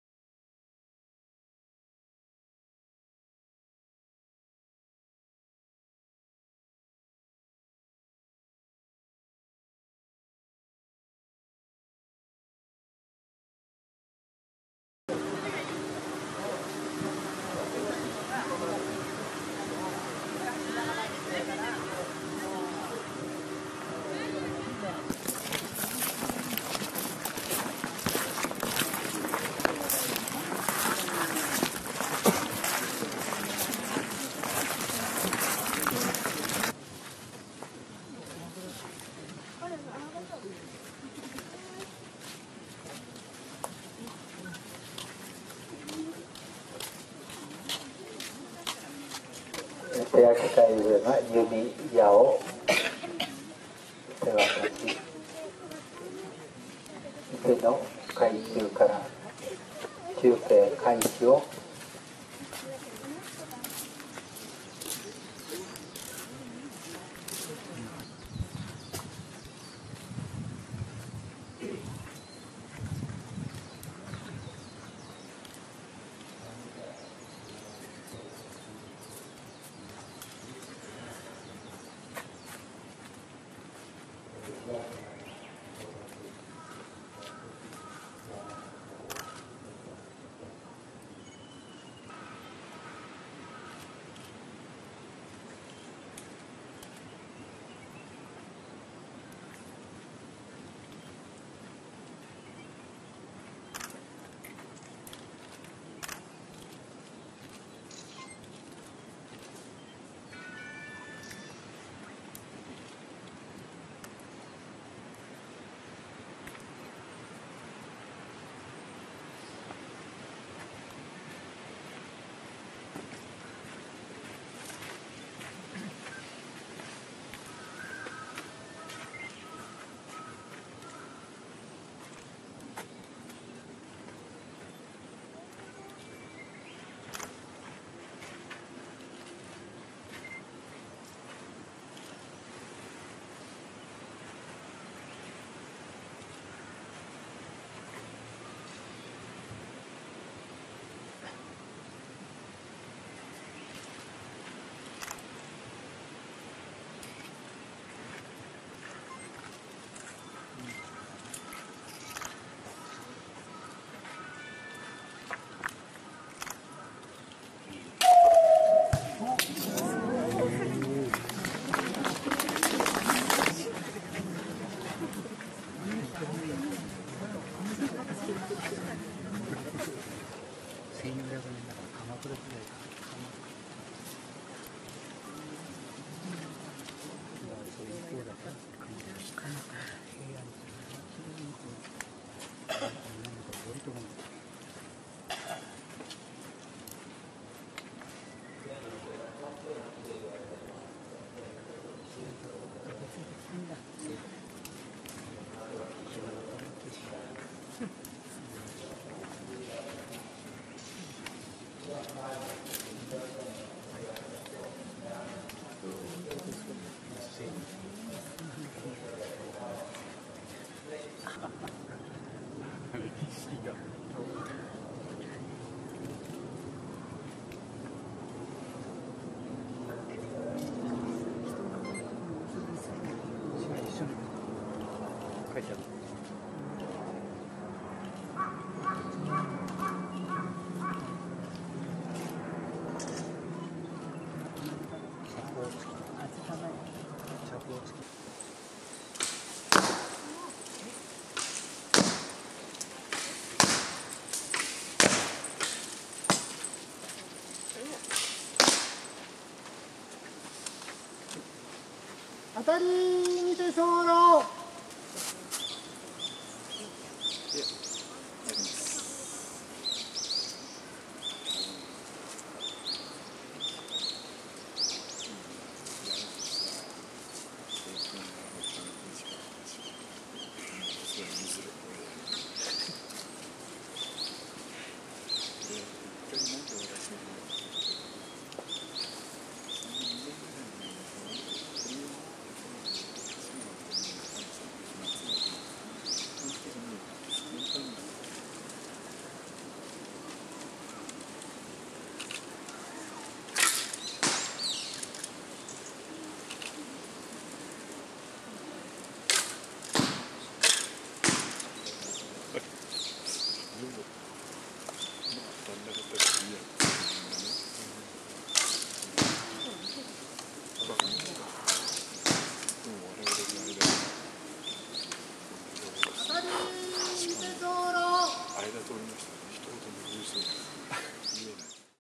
鹿島神宮 小笠原流百々手式.mp4
鹿島神宮で奉納された、小笠原流百々手式の様子です。
絵巻物語りのような衣装を身にまとい、弓を打つ姿が印象的です。